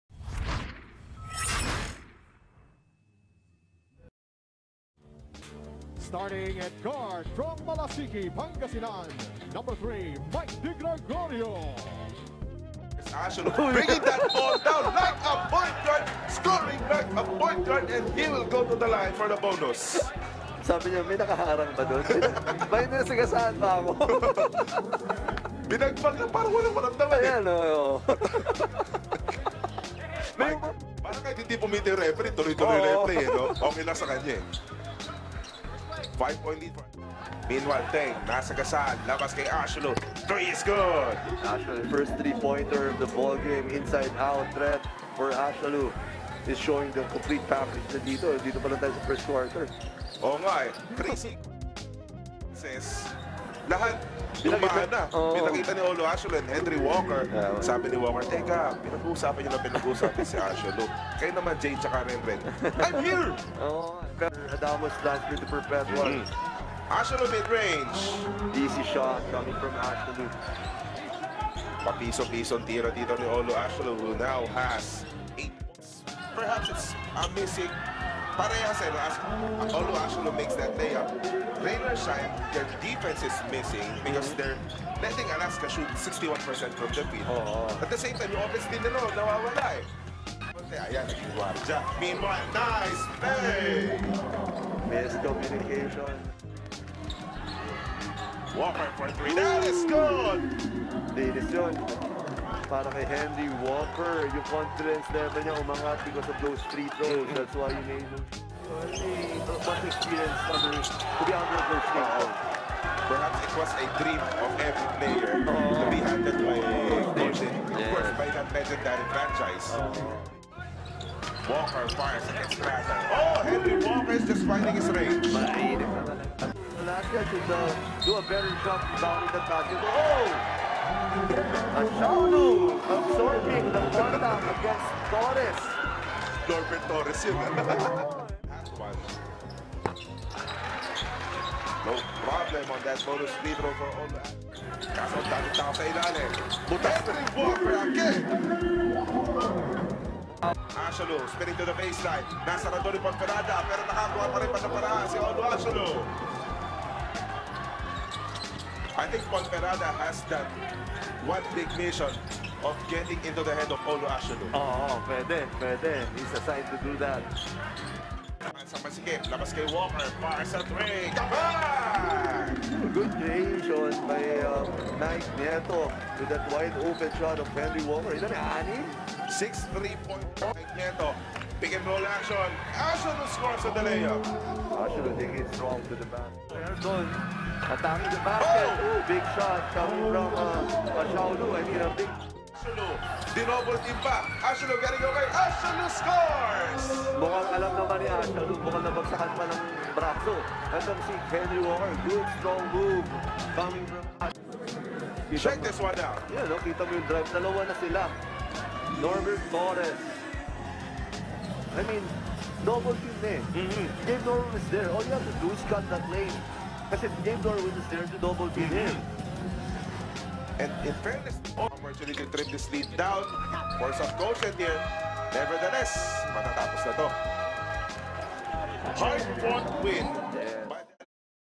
Olu Ashaolu vs Henry Walker Full Duel Highlights | 2-17-22 | Ashaolu 23 pts | Walker 25 pts
2021 PBA Governors Cup Import Duel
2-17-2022
Akaska Aces 80 | Rain or Shine Elasto Painters 74 | FINAL